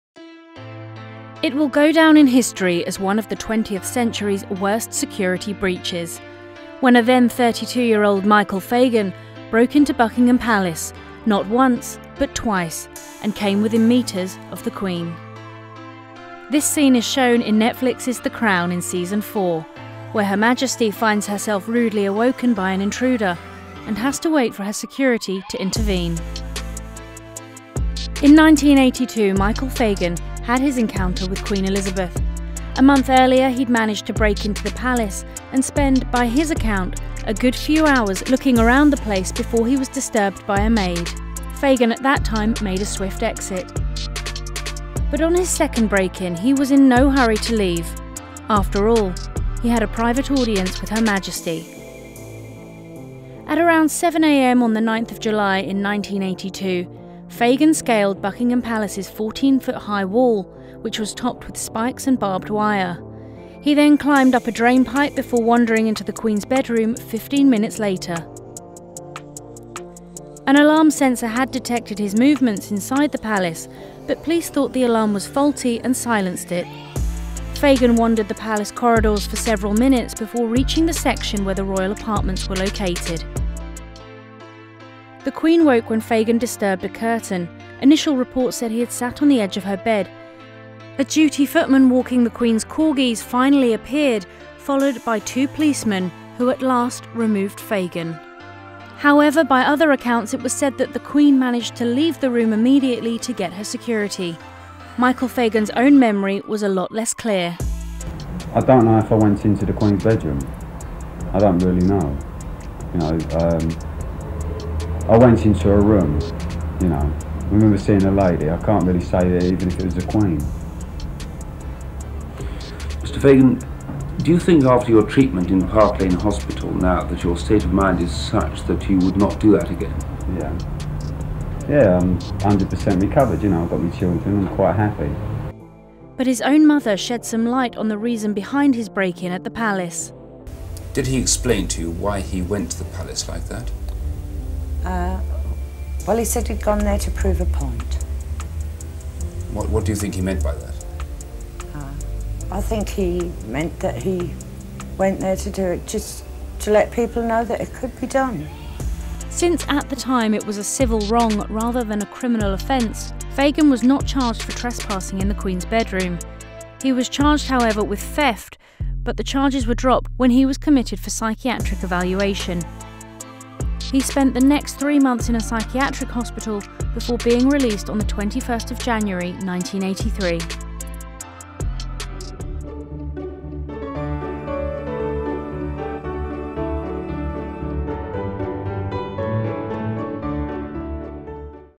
‘The Crown’ Season 4: Intruder at Buckingham Palace
One summer’s morning in 1982 Queen Elizabeth II awoke startled when she realised an intruder was in her room.

The intruder was Michael Fagan, an unemployed tradesman who wanted to have a chat with Her Majesty.

What transpired between the Queen and Fagan has since gone down in history as one of the most dramatic royal security breaches on record.

Nearly four decades later, no one really knows what exactly went on - except for Fagan and the Queen. Now the episode has been dramatised for a Season 4 episode of Netflix’s ‘The Crown’.